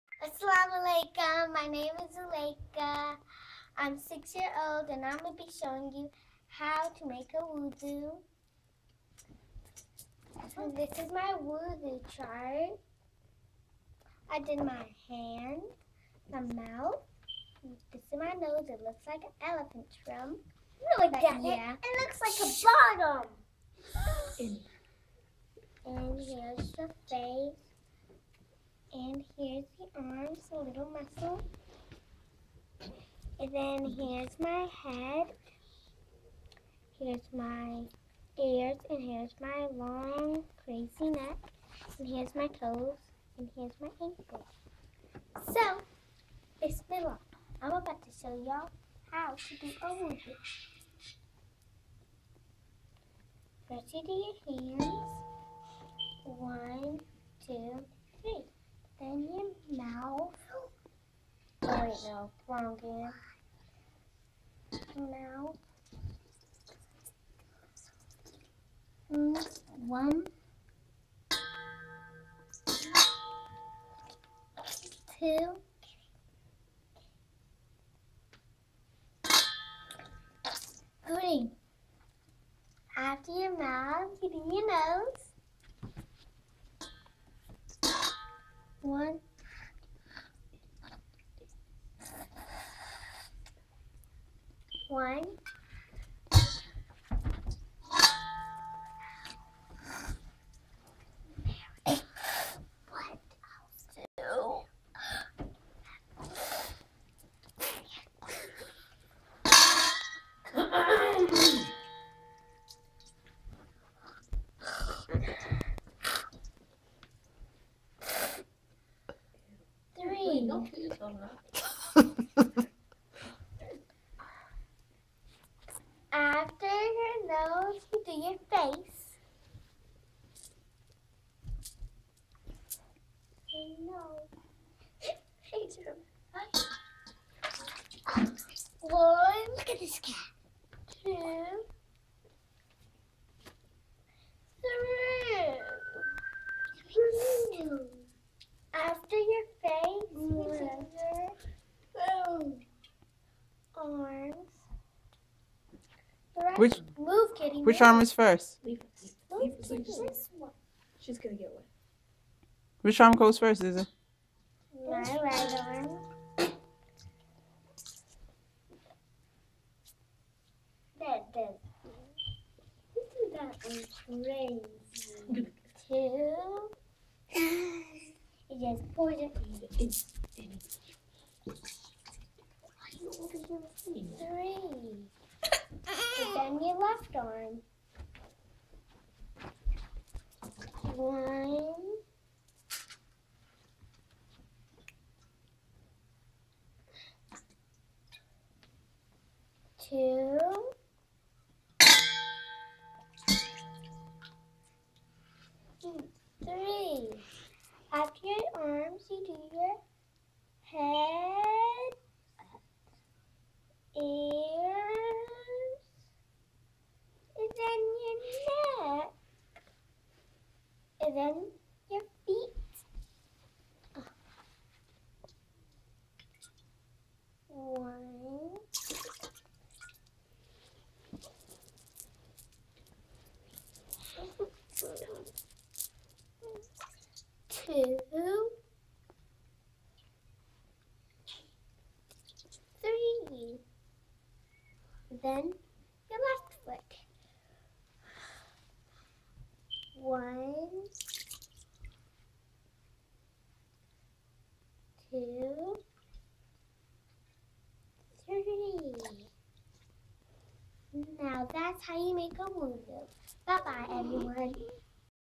Zulaikah teaches us how to make a wudu
Zulaikah our 6 year old teaches us to make a wudu